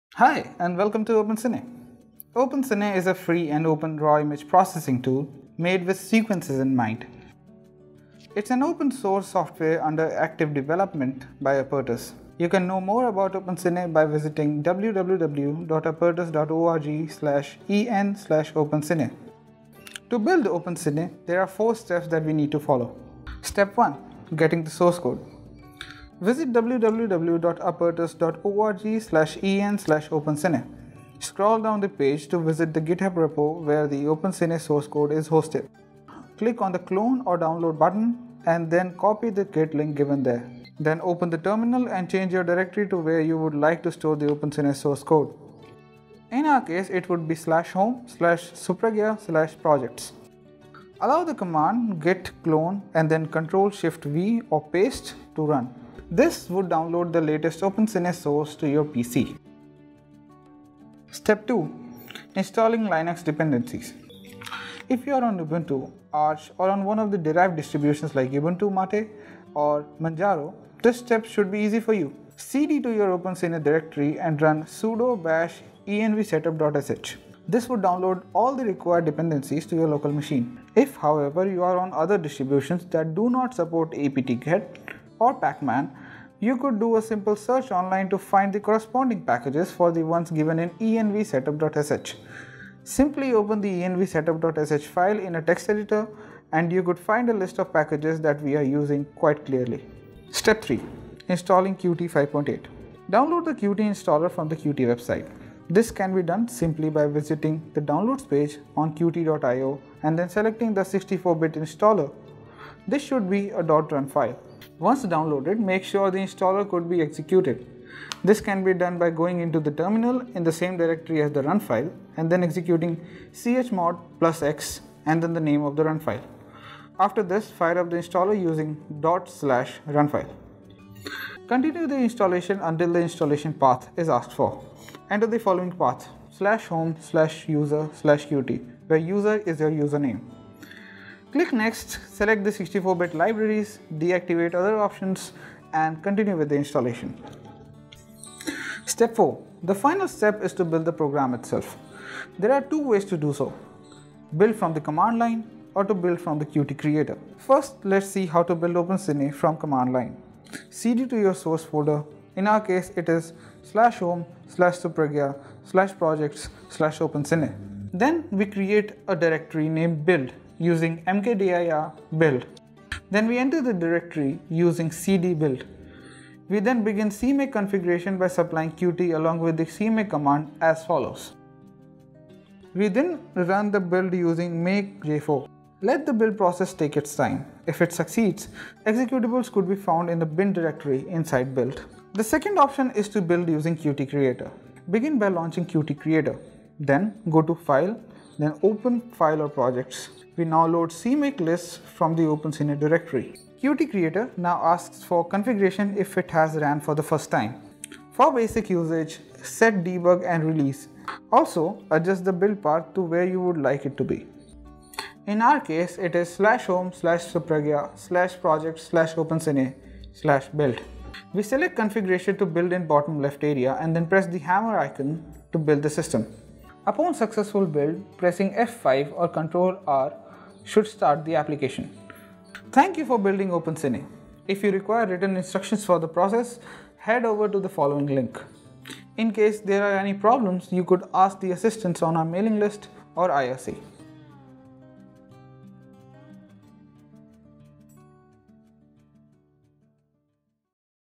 Opencine build instructions
OpenCine is a free RAW video processing suite under active development.